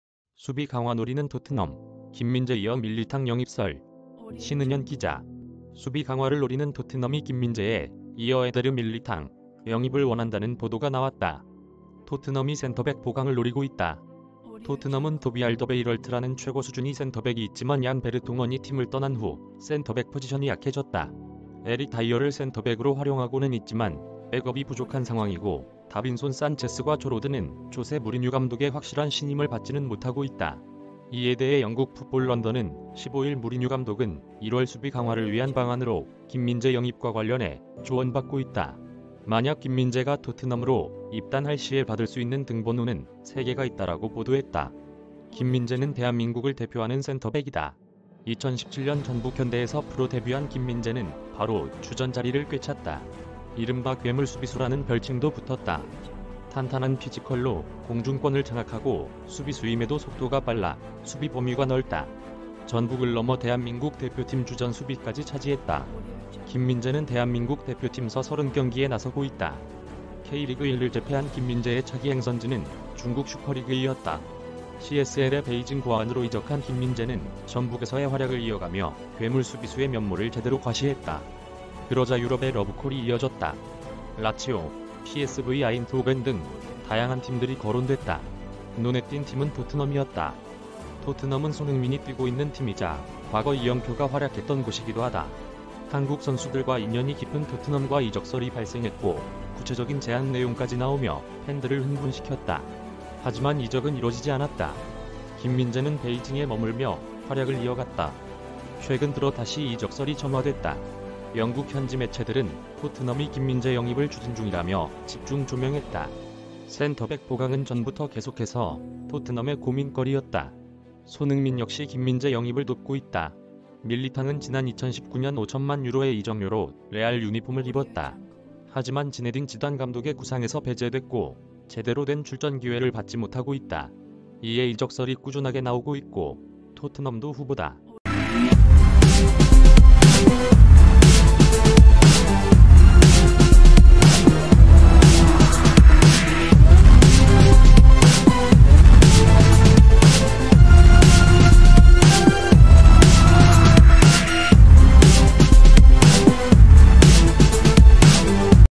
'수비 강화' 노리는 토트넘, 김민재 이어 밀리탕 영입설 - 해외 반응
[인터풋볼] 신은현 기자= 수비 강화를 노리는 토트넘이 김민재에 이어 에데르 밀리탕 영입을 원한다는 보도가 나왔다. 토트넘이 센터백 보강을 노리고 있다. 토트넘은 토비 알더베이럴트라는 최고 수준의 센터백이 있지만 얀 베르통언이 팀을 떠난 후 센터백 포지션이 약해졌다. 에릭 다이어를 센...